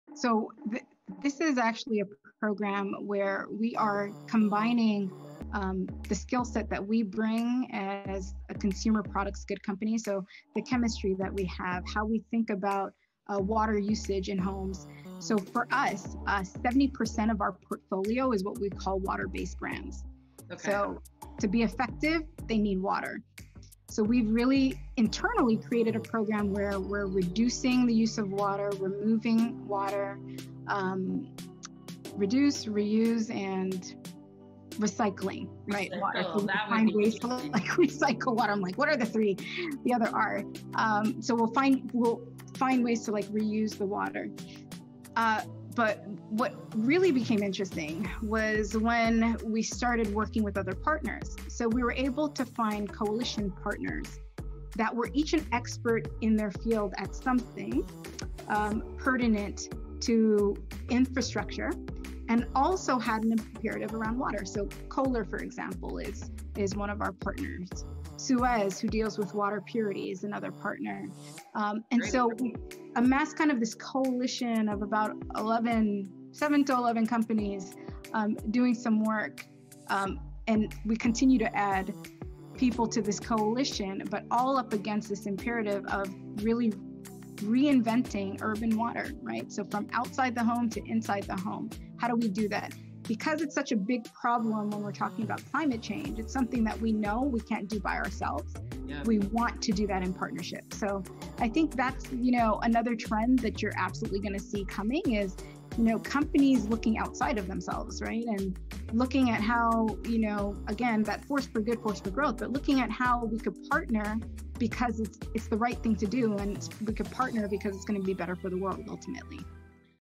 Tanya Stephens, Director, Sr. Innovation & Technology Leader at Procter + Gamble - Water
Tanya Stephens, Director, Sr. Innovation & Technology Leader at Procter + Gamble. Understanding emerging technologies like blockchain is key to understanding the complex web of financial services that influence key decisions in life and business. Tanya talks about how government and business can work together to advance emerging technologies. When you give people tools, they will solve some of the problems happening. One of the ways you do that is by telling stories about how to empower people. We have to do it in partnership.